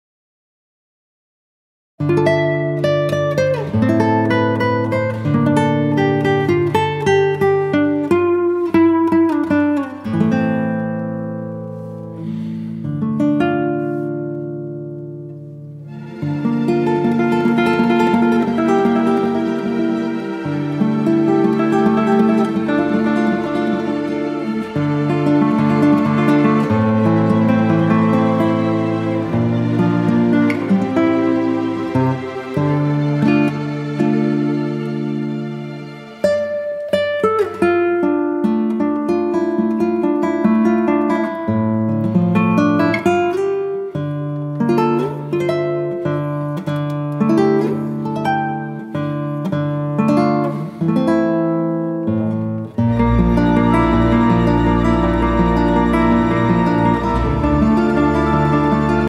Florence, Italy. Cathedral Santa Maria del Fiore. Panoramic view of the city. Summer. Evening, Aeria
Florence, Italy. Cathedral Santa Maria del Fiore. Panoramic view of the city. Summer. Evening, Aerial View